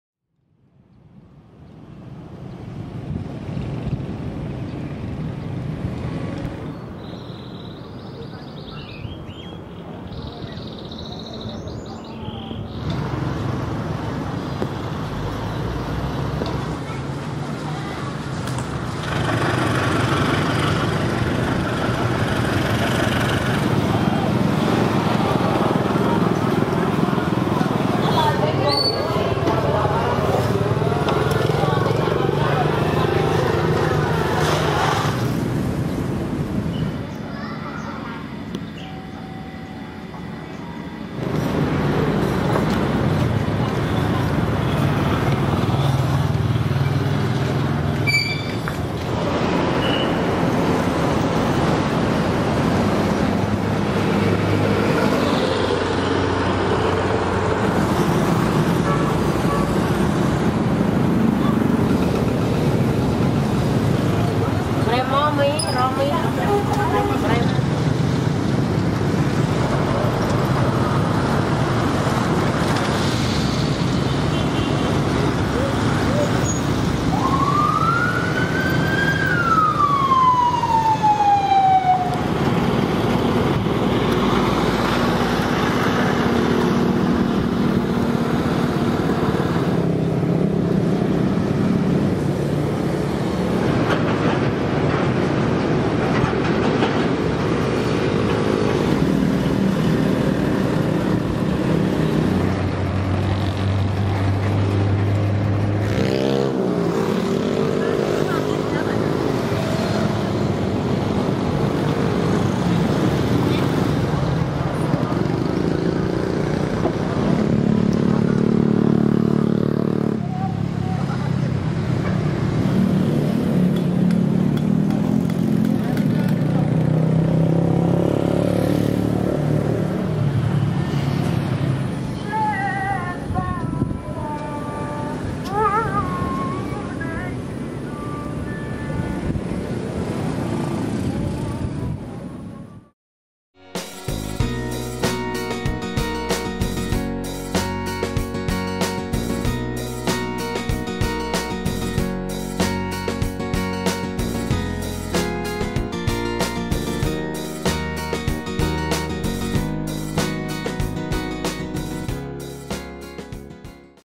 Semarang, Java, Indonesia
Semarang is the largest city in central Java, and a major port in the Dutch colonial era.